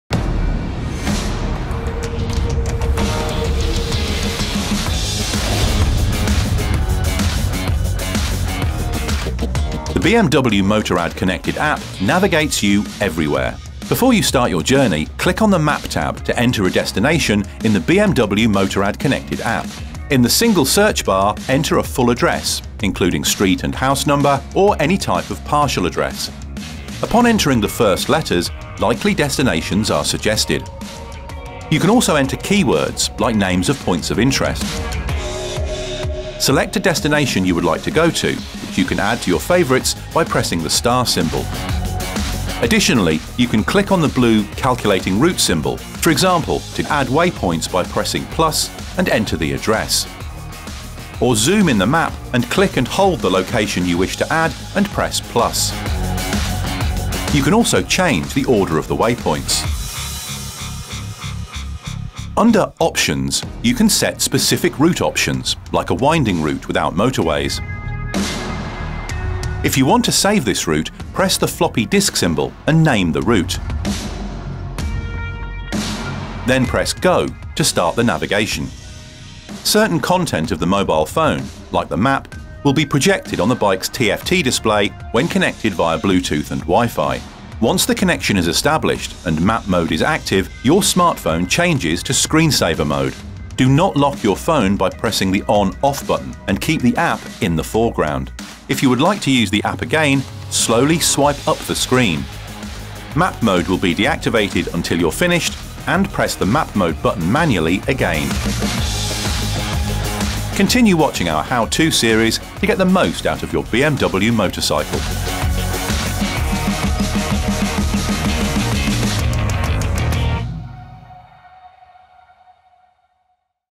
BMW Motorrad Australia | Navigate your journey with the BMW Motorrad Connected App
Learn in this video how to navigate with the BMW Motorrad Connected App and your BMW motorcycle.

Before you start your journey, click on the map tab to enter a destination in the BMW Motorrad Connected app. In the single search bar, enter a full address, including street and house number, or any type of partial address. Upon entering the first letters, likely destinations are suggested. You can also enter key words like names of points of interest. Select a destination you would like to go to, which you can add to your favourites by pressing the star symbol. Additionally, you can click on the blue ‘Calculating route’ symbol, for example to add waypoints by pressing ‘+’ and enter the address. Or zoom in the map, click and hold the location you wish to add and press ‘+’. You can also change the order of the waypoints. Under ‘OPTIONS’ you can set specific route options like a winding route without motorways. If you want to save this route, press the ‘Floppy disk’ symbol and name the route. Then press “GO” to start the navigation. Certain content of the smartphone, like the map, will be projected on the bike’s TFT Display when connected via Bluetooth and Wi-Fi. Once the connection is established, and map mode is active, your smartphone changes to screensaver mode. Do not lock your phone by pressing the on / off button and keep the app in the foreground. If you would like to use the app again, slowly swipe up the screen. Map mode will be deactivated until you are finished and press the map mode button manually again.

Check out the other videos in the BMW Motorrad How-To Series and be sure to leave us your feedback in the comments below.